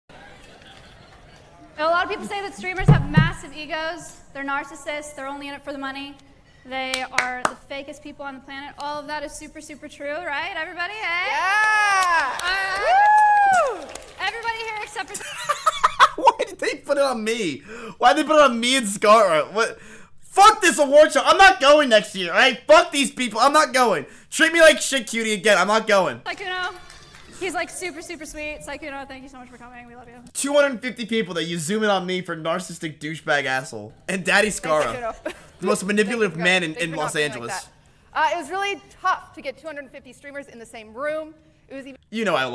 Mizkif isnt going to The Streamer Awards next year...
While reacting to The Streamer Awards, Mizkif criticizes a coy move by QTCinderella, Maya, and production. He didn't appreciate the implication of character by him and Scarra. For example, they could have literally panned the camera to anyone else, like Ludwig, Pokimane, Disguised Toast, QuarterJade, Masayoshi, Lily Pichu, Michael Reeves, Esfand, Cyr, Fuslie, Xqc, Jerma, and countless others who were also at the event.